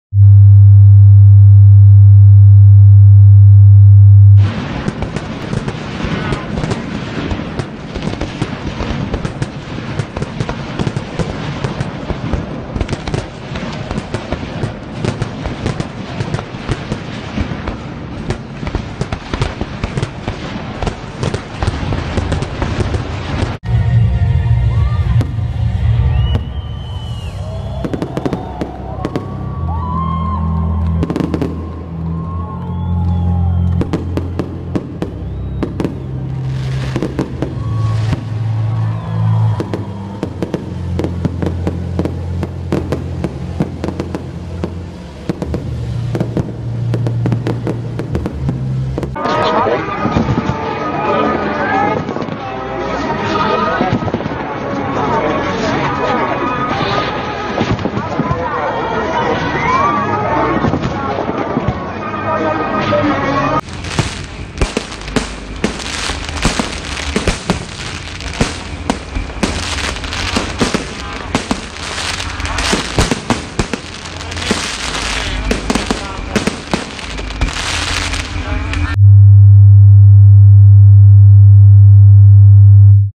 RORSCHACHWERKS #8
Rorschach Werks #8
My series of psychedelic, celebratory, mirrored fireworks.